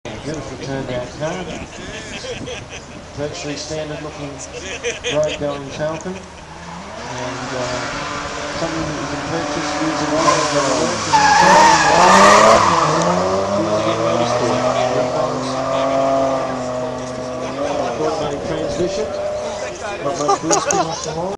Mt Gambier Blue Lake Hillclimb 2006
Footage from '06' Blue Lake Hillclimb. The first of hopefully a annual event.
A toyota supra drifting at the top section of the valley lake.